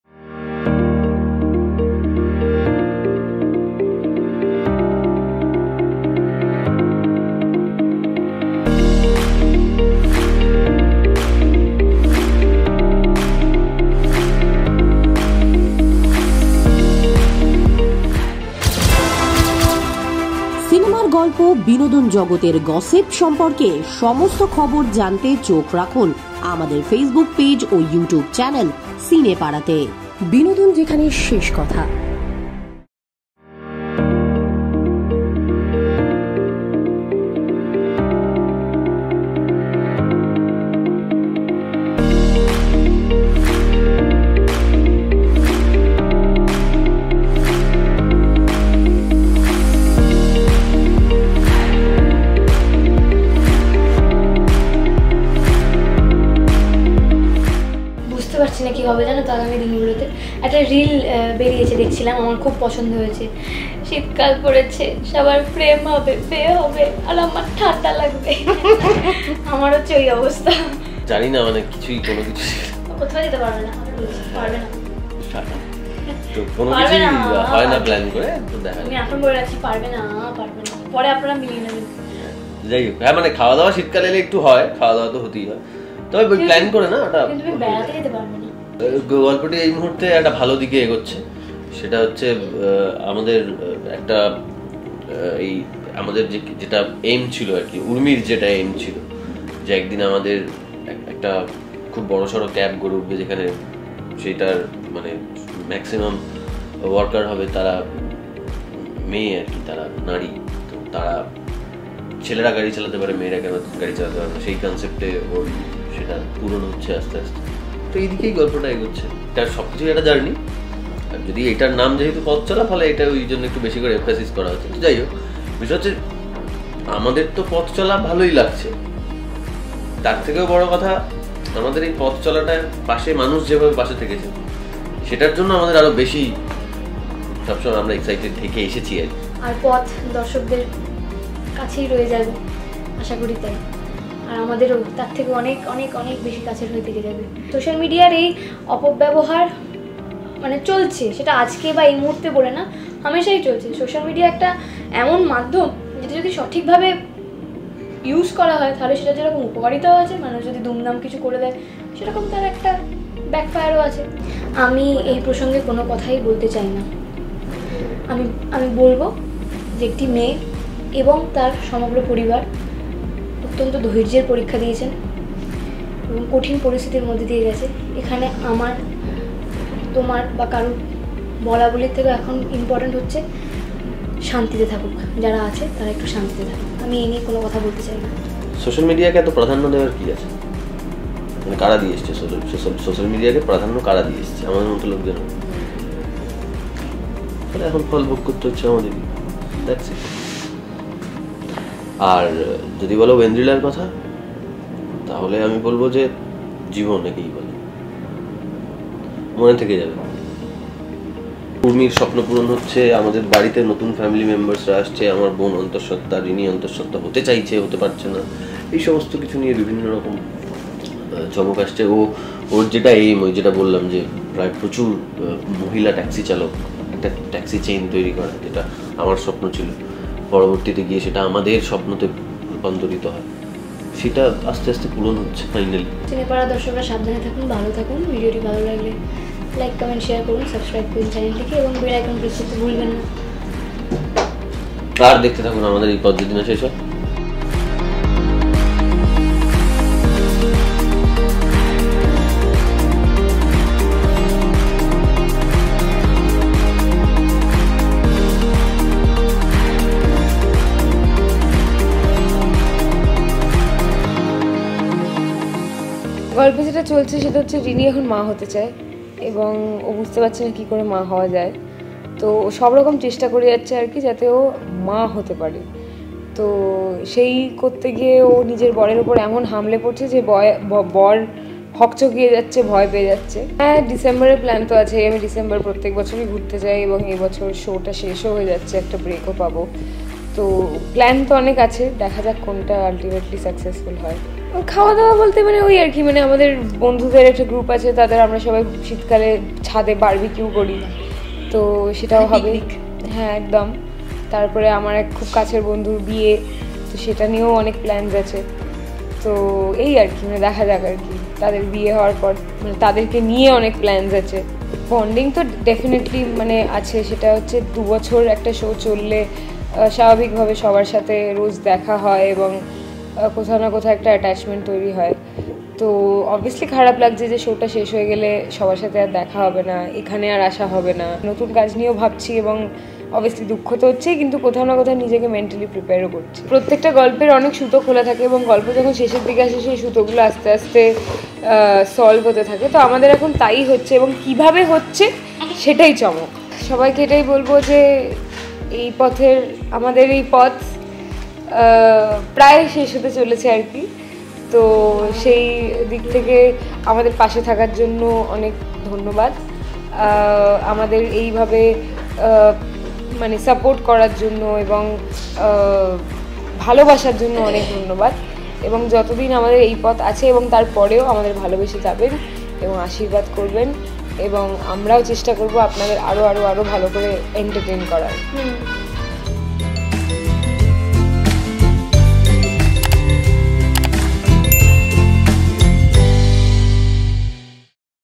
'এই পথ যদি না শেষ হয়' ধারাবাহিকের নতুন চমক নিয়ে কী বললেন উর্মি,সার্তকি এবং রিনি?|Zee Bangla|Exclus
'এই পথ যদি না শেষ হয়' ধারাবাহিকের নতুন চমক নিয়ে কী বললেন উর্মি,সার্তকি এবং মিশমি?